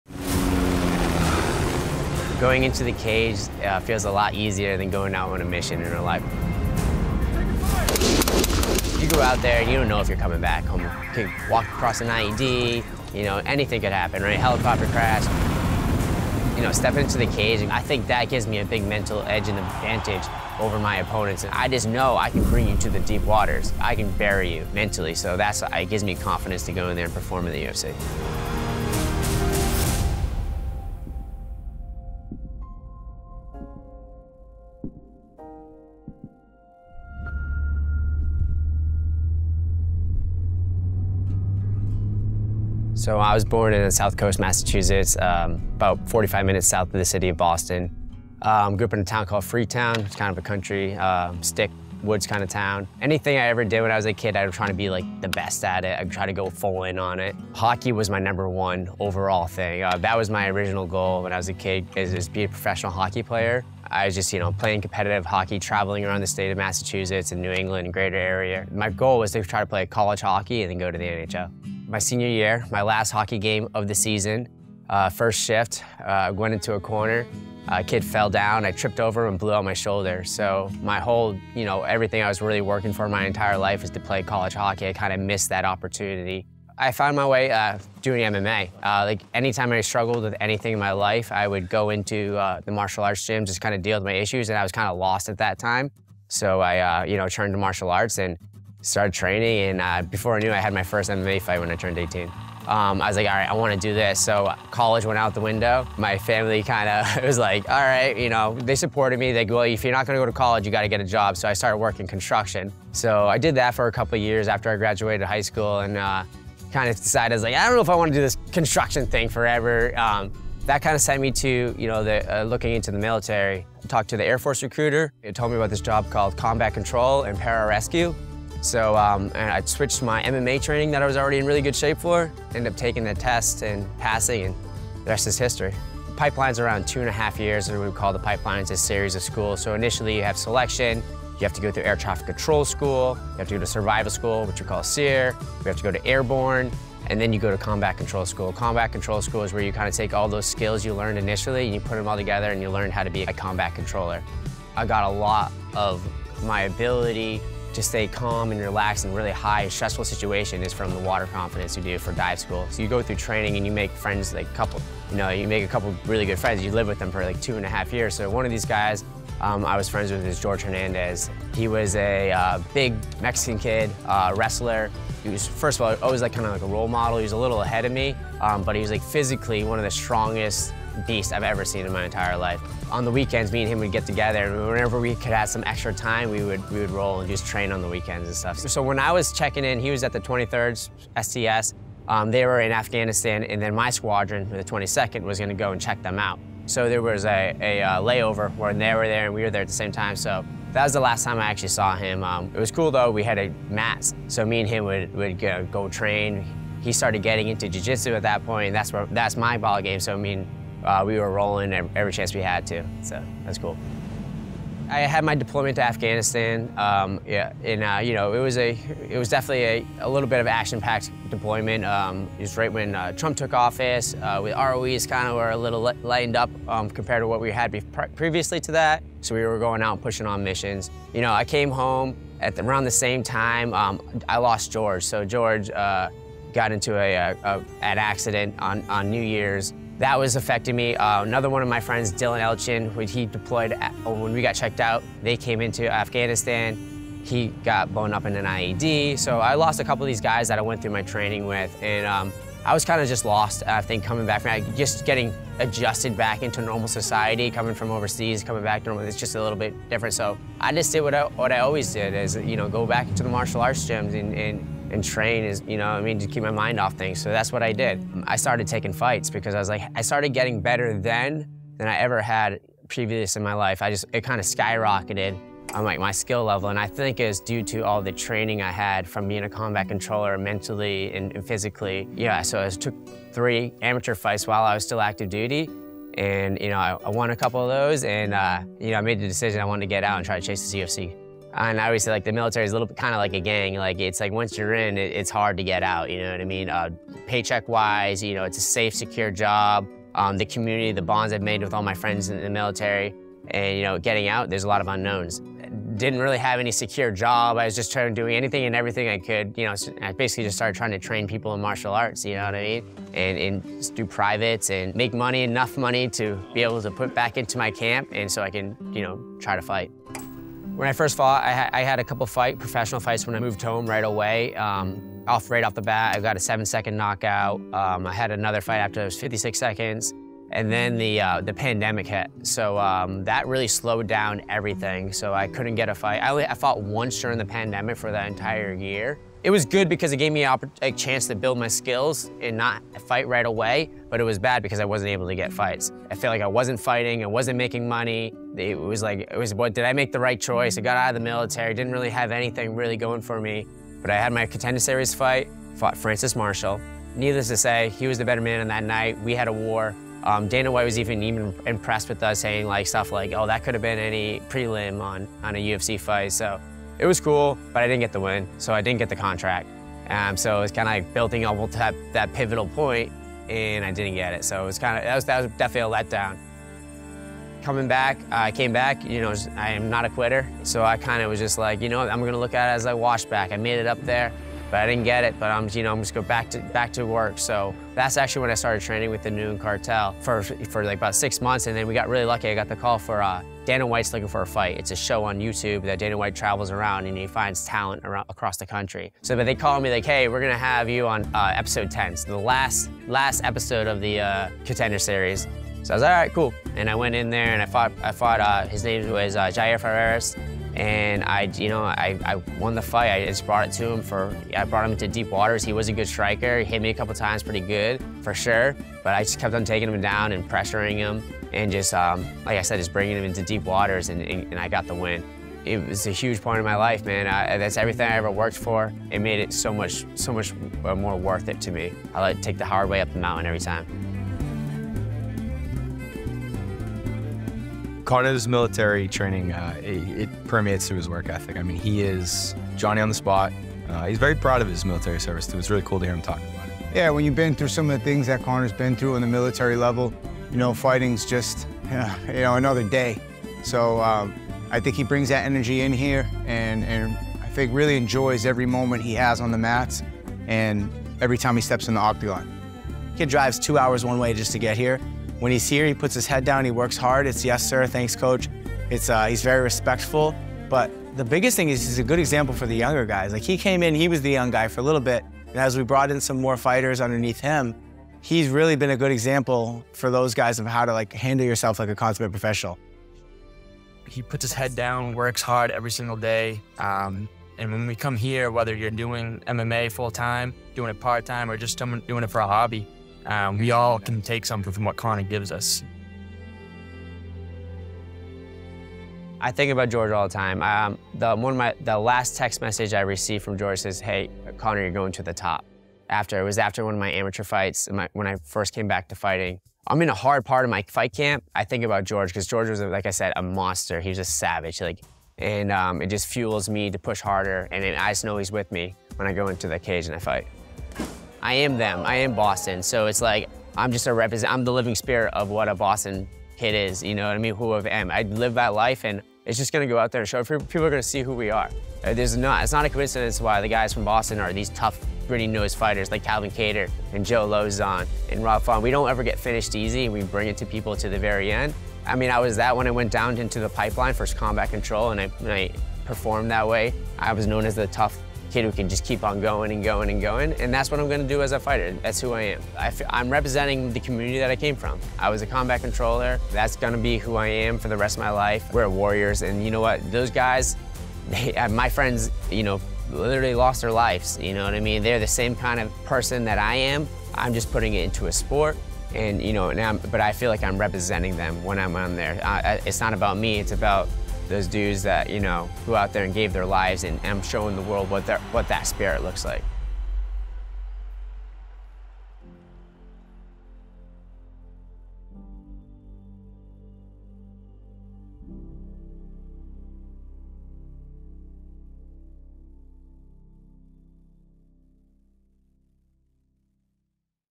BRCC Presents - Connor "The Controller" Matthews
From Combat Controller to UFC Fighter, Connor Matthews embodies American bada**! Holding a professional record of 7-1-0, Connor's path to the UFC, sealed by his victory in Dana White's Contender Series, is hard fought and well earned.